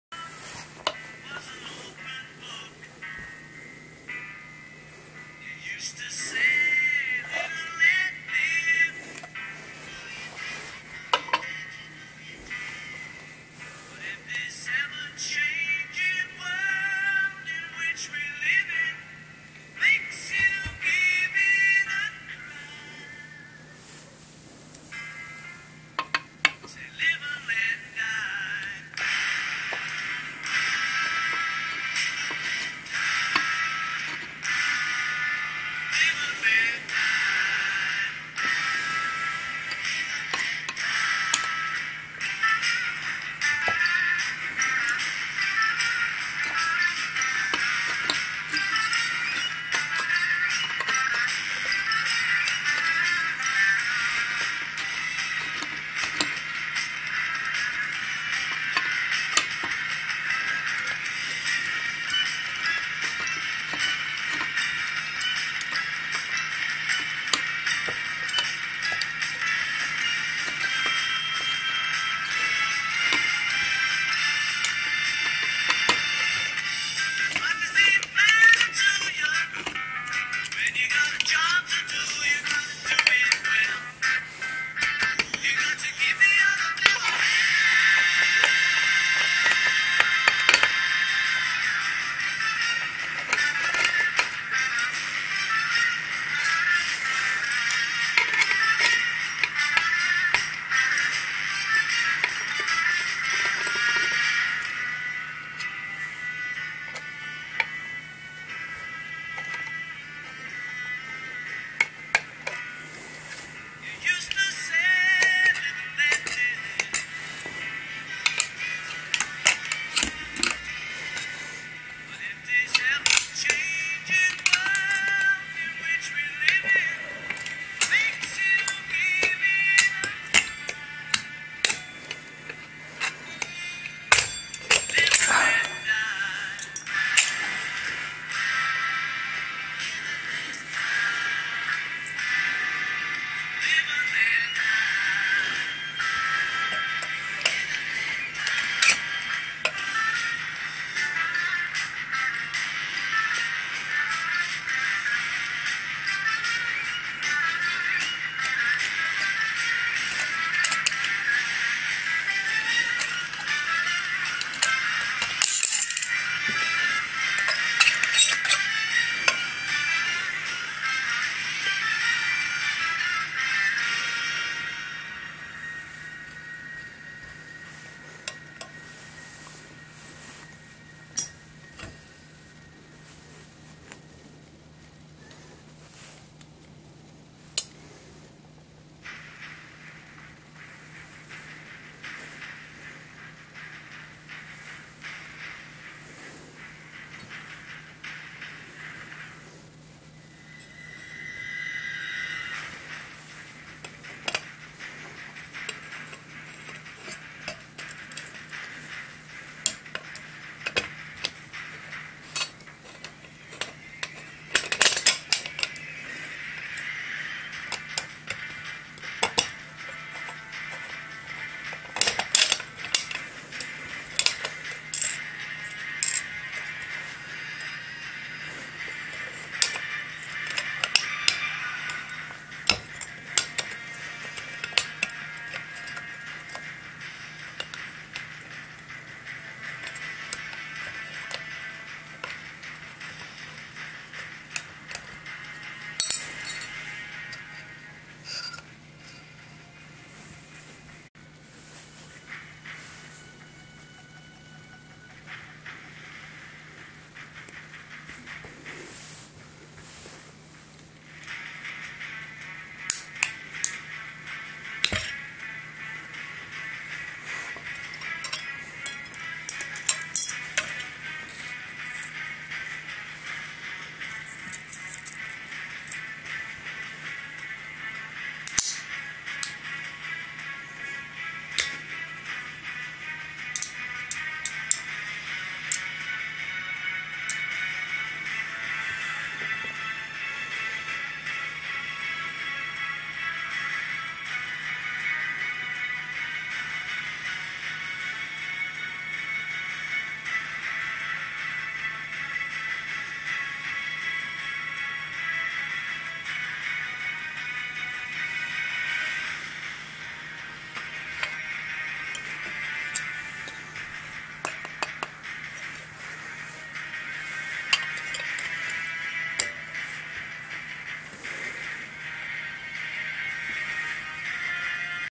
Deconstructing A Hard Drive
Deconstructing a hard drive in HD and taking off the rare earth magnets (neodymium magnets) which was really hard
I will try to make a homemade electric generator by using these super powerful magnets and long thin-coated wire